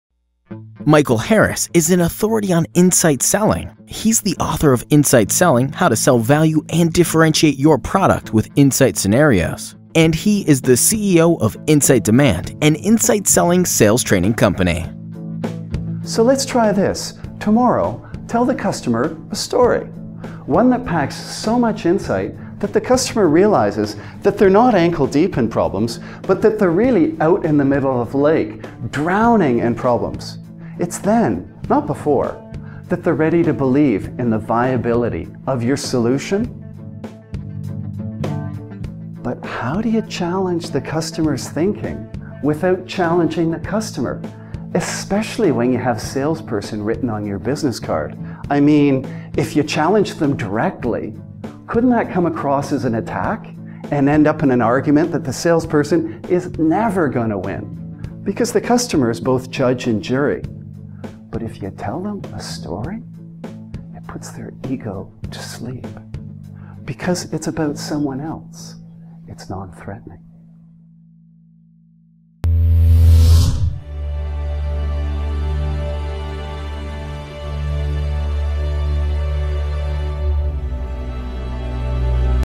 Speaking Reel, Michael Harris, CxO, Insight Demand
Michael Harris will show your salespeople how how to inspire customers to buy with Insight Selling.

Because insight scenarios help customers to realize that they are rationally and emotionally drowning in problems, your salespeople will finally be able to rescue more customers with your product.